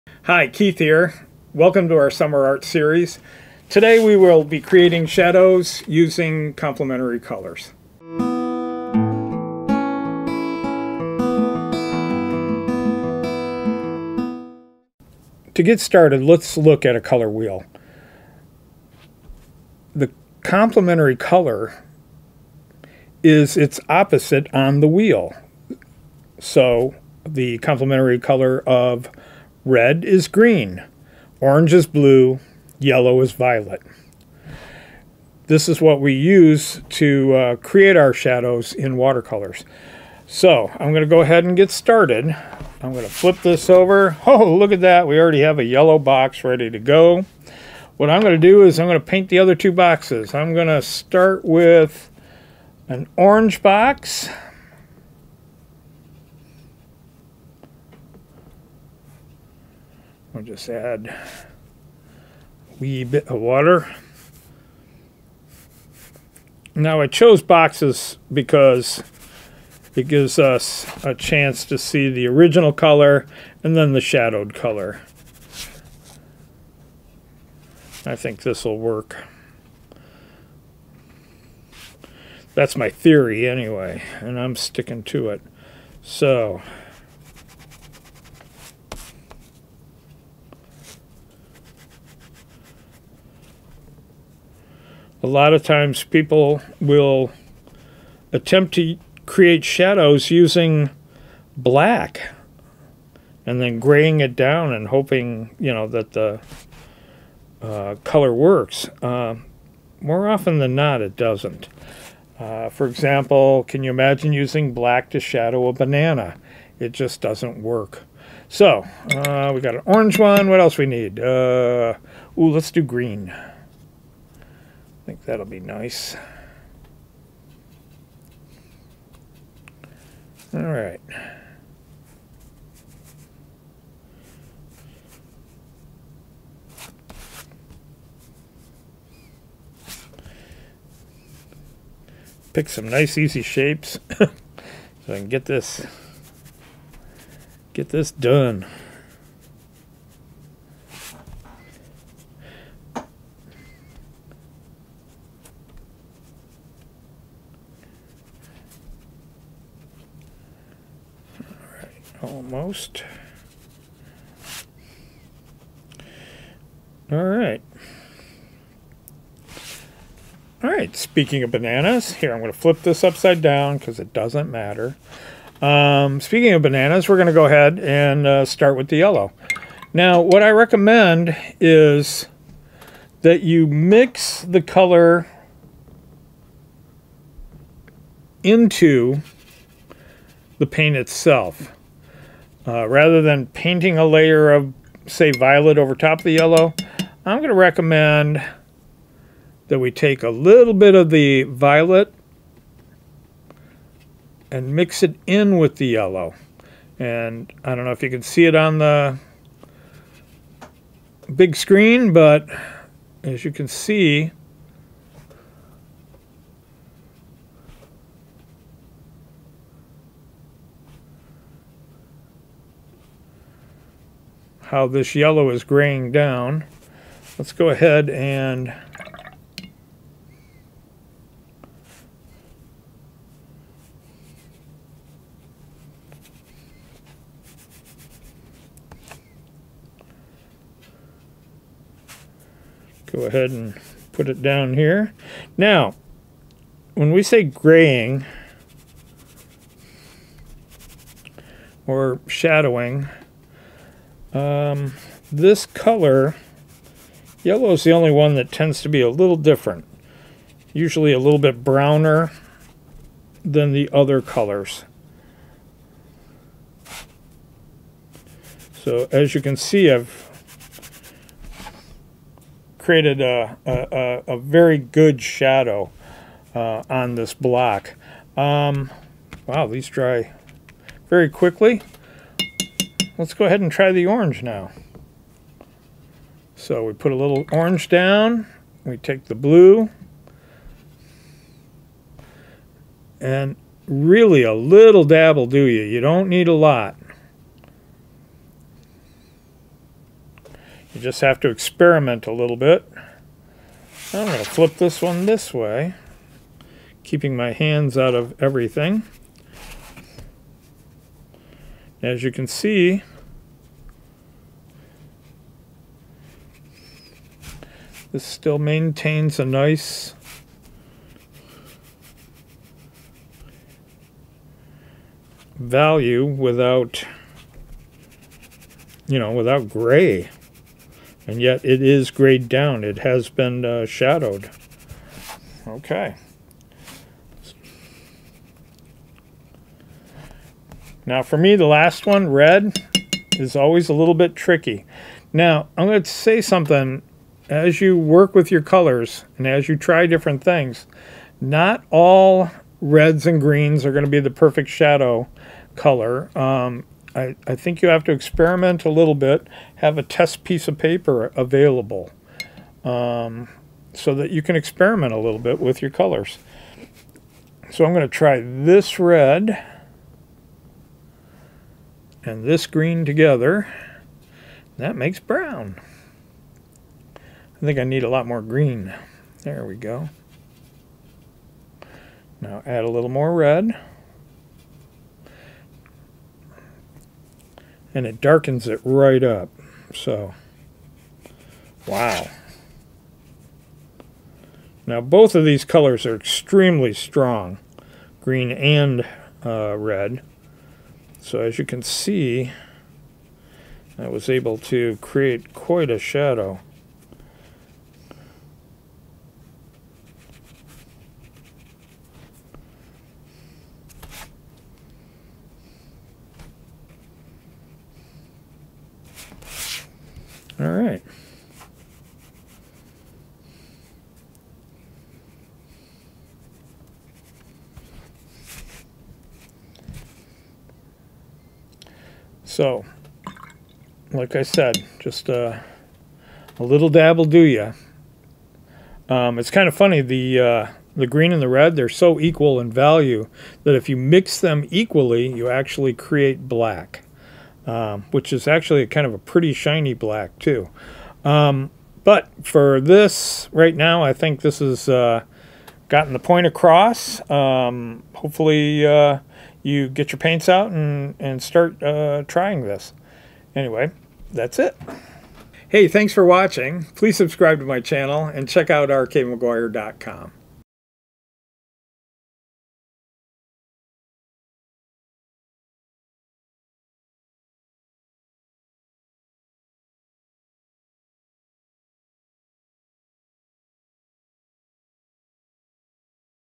How to Paint Shadows using Complementary Colors | Watercolor | R K McGuire
In this video Keith shows how to create shadows in your watercolors using complementary colors.